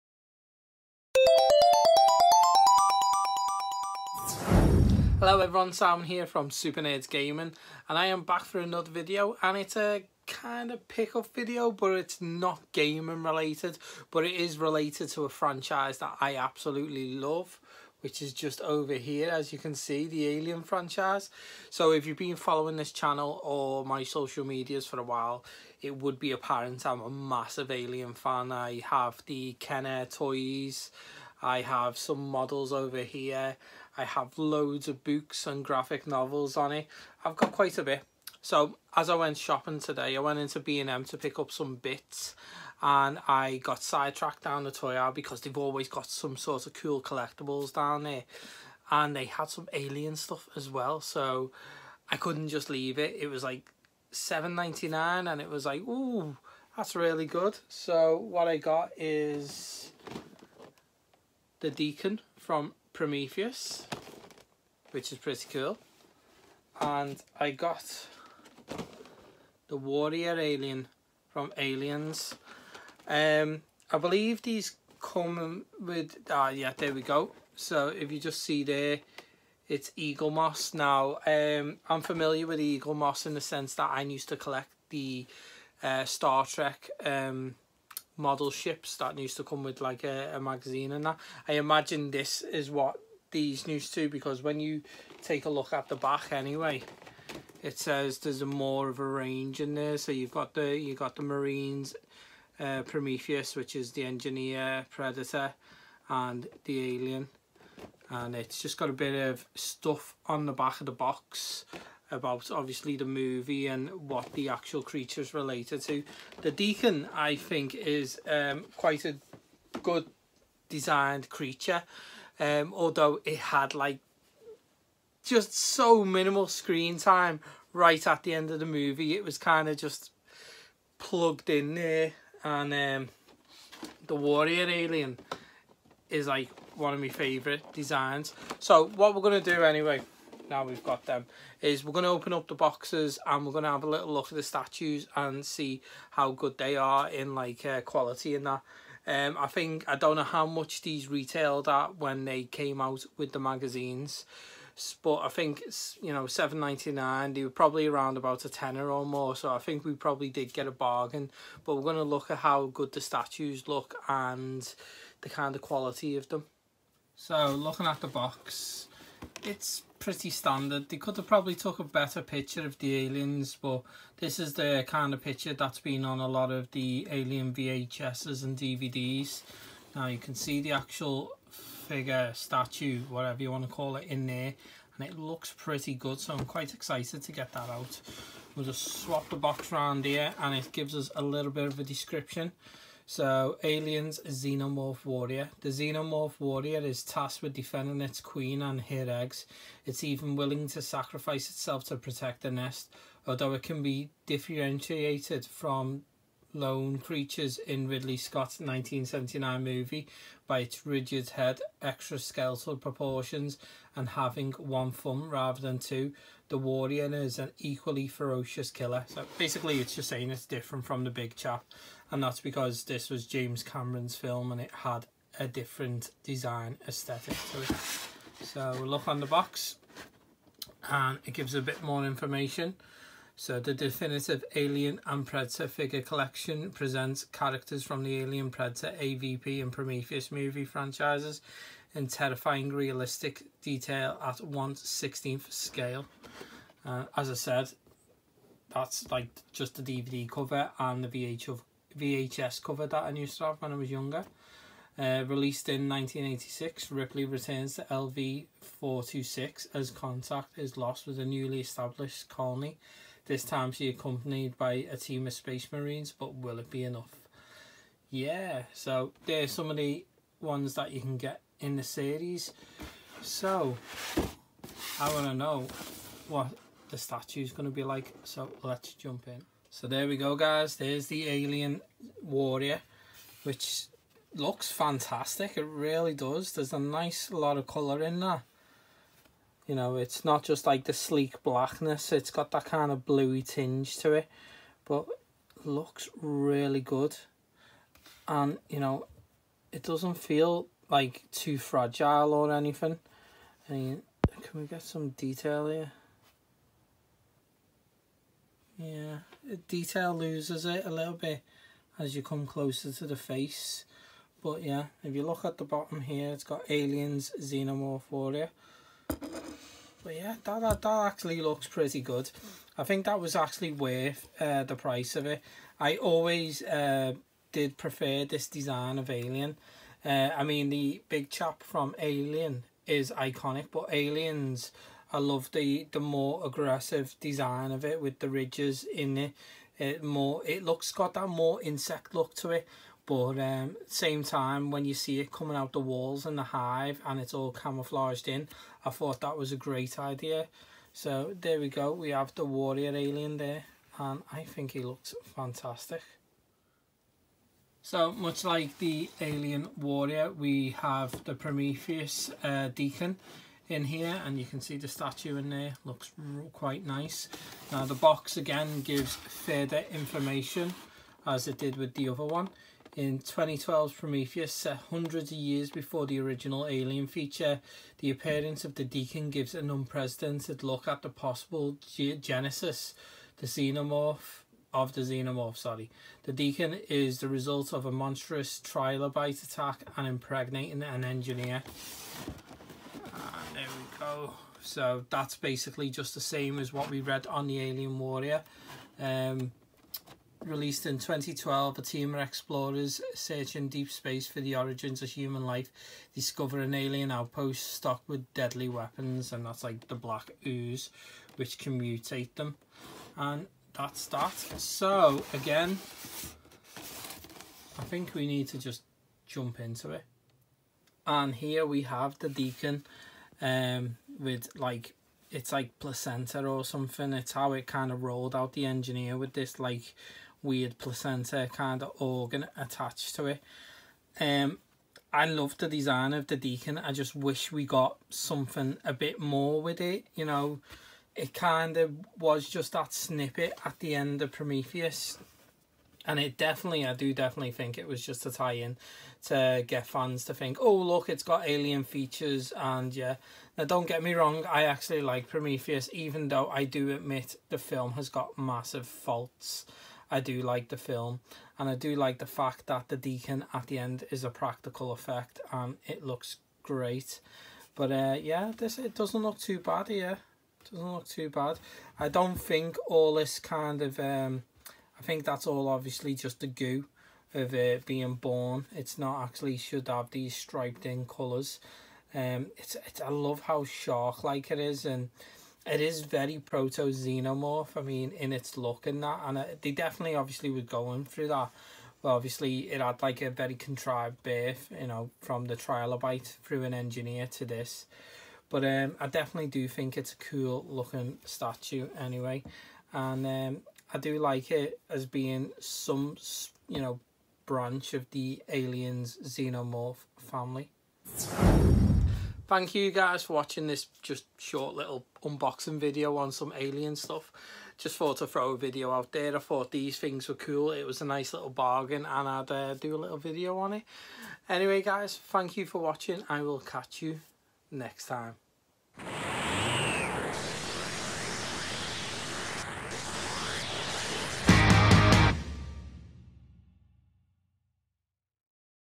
Aliens Eaglemoss statue unboxing
Join Simon as he unboxes some awesome Alien statues